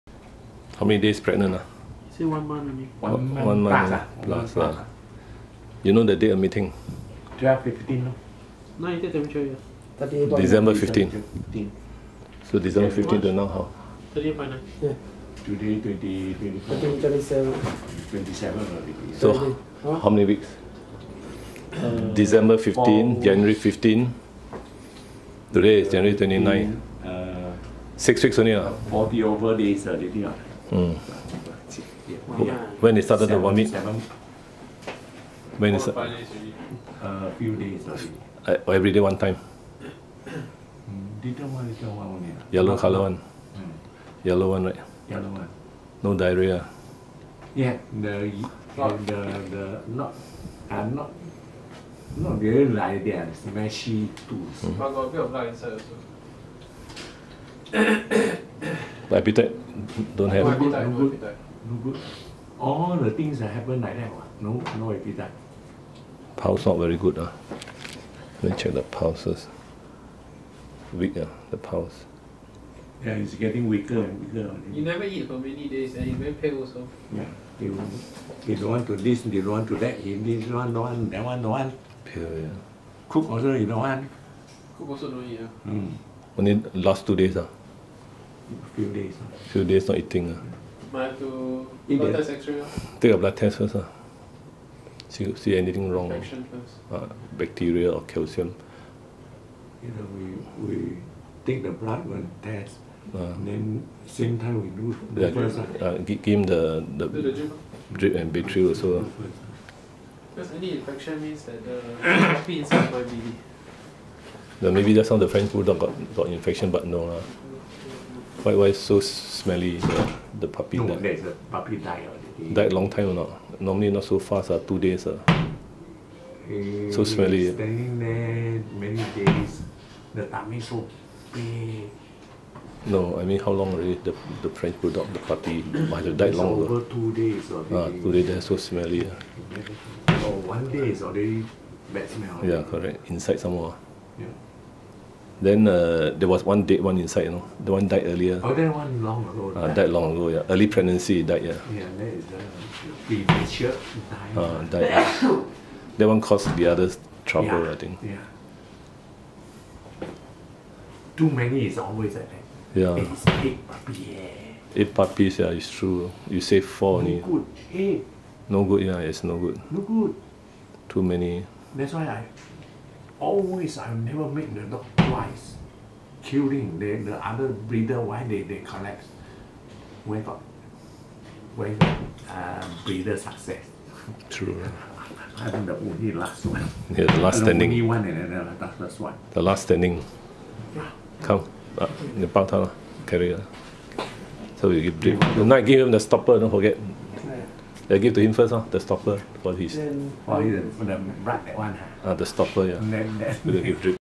A pregnant corgi lost appetite and vomited for a few days
The Corgi is around 42nd-day pregnant. She had lost appetite and vomited. This is likely eclampsia. Calcium IV drip is given.